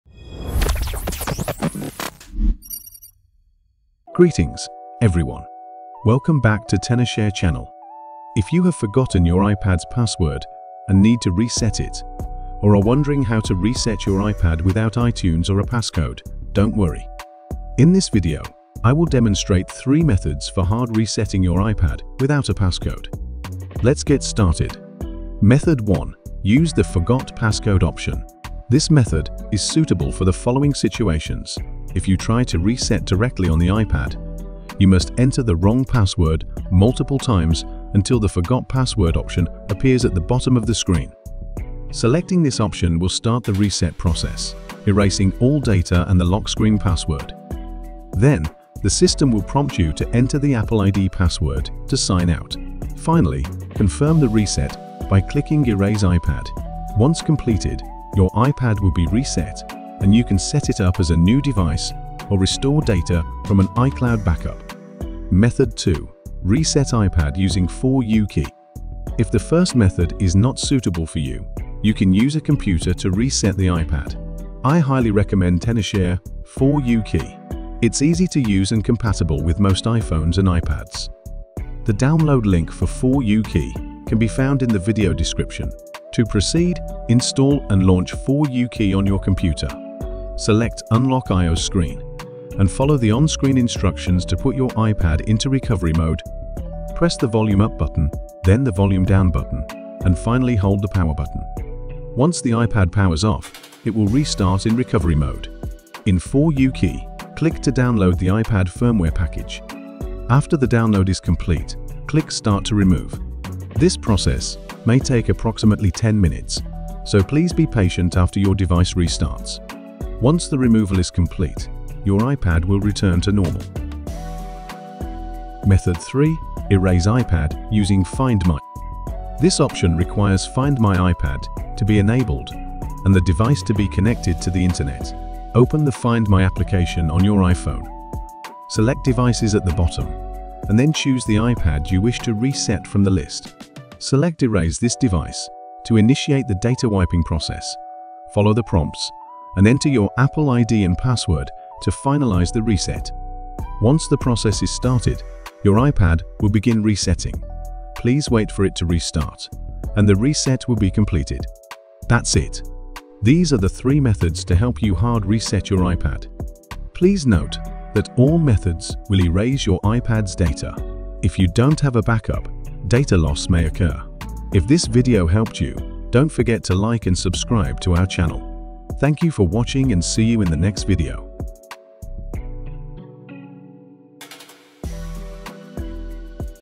How to Reset iPad Without Passcode (Step by Step Guide 2025)
This video is a tutorial that explains how to reset an iPad without a passcode using three simple methods. It first introduces the “Forgot Passcode” option, where entering the wrong password several times triggers an on-screen reset process that requires Apple ID verification. The second method uses Tenorshare 4uKey, a computer tool that allows users to remove the lock screen and reinstall the firmware, restoring the iPad to normal. The final method explains how to erase the iPad via Find My, which lets users remotely reset the device if Find My iPad is enabled. The video ends with a reminder that all methods erase data, so backups are recommended before resetting.

⏱ *TIMESTAMP*
0:00 intro
0:25 Use the Forgot passcode option
1:05 Reset iPad using 4uKey
2:05 Erase iPad Using Find My

Method 1: Use the “Forgot Passcode” Option
When you enter the wrong passcode several times, the iPad will display the iPad Unavailable or Security Lockout screen. Tap Forgot Passcode?, then enter your Apple ID and password to erase and reset the device. This method works directly on the iPad without needing a computer.
Method 2: Use Tenorshare 4uKey
Download and install Tenorshare 4uKey on your computer. Connect the locked iPad, launch 4uKey, and select Remove Passcode. The software will download the firmware and erase the lock screen, restoring the device to factory settings. It’s a simple way to reset an iPad if you forgot your passcode or don’t have access to iTunes.
Method 3: Use Find My iPad
If Find My iPad is enabled, go to iCloud.com/find on another device or computer, sign in with your Apple ID, select your iPad from the device list, and click Erase iPad. This remotely deletes all data and unlocks the iPad, allowing you to set it up as new.

Free Image Background Remove: pixpretty.tenorshare.ai